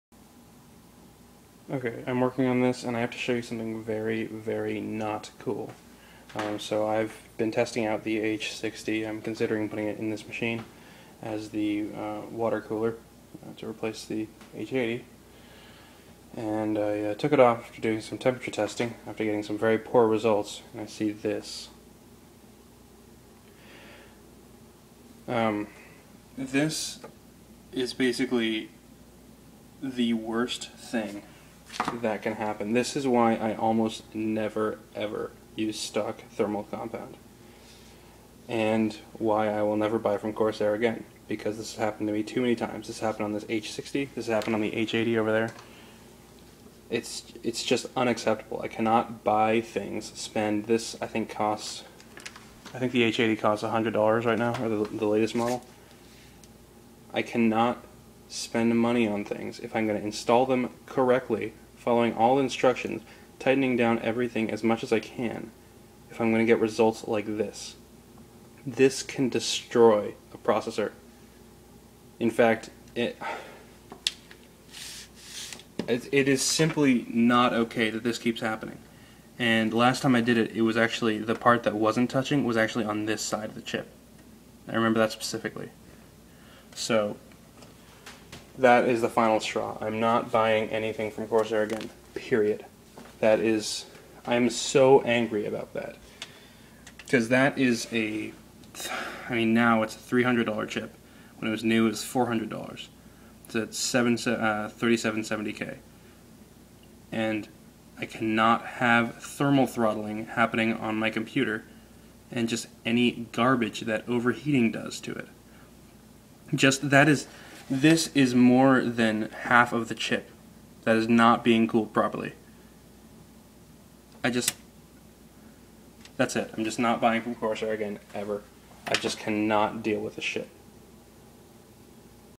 Corsair H60 Complaints
Here comes the tirade of corsair fanboys...

Ok, look. This is the third out of four corsair products that I've purchased to not work properly. All of the coolers that I have purchased from corsair have either broken or have had a problem similar to this. Improper cooling is the one thing that actually physically damages processors. I can not have my $300 CPU be destroyed because of a poor quality third party cooler.